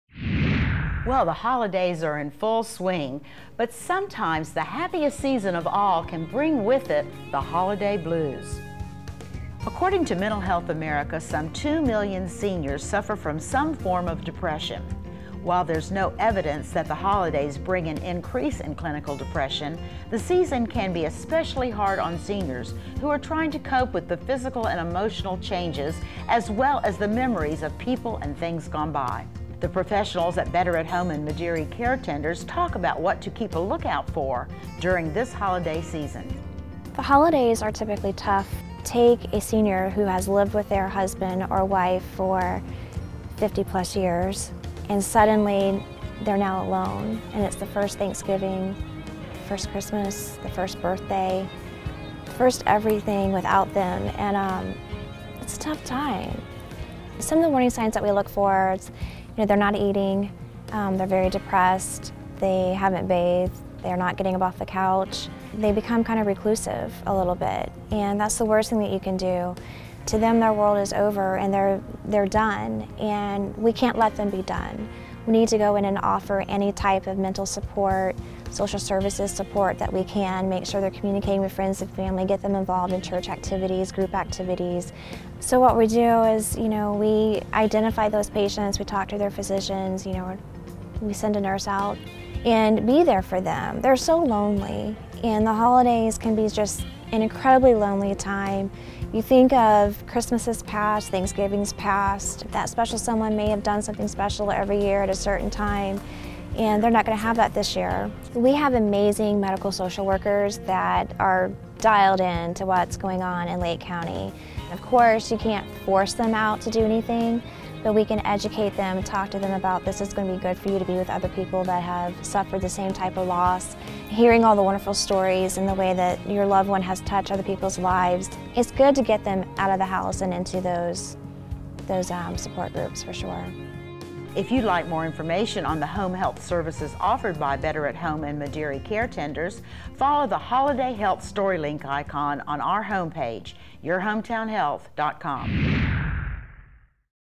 Better at Home/Mederi Caretenders: Holiday Health
While there's no evidence that the holidays bring an increase in clinical depression, the season can be especially hard on seniors who are trying to cope with physical and emotional changes as well as the
memories of people and things gone by.

The professionals at  Better At Home / Medieri Caretenders  talk about what to keep a lookout for, during this holiday season.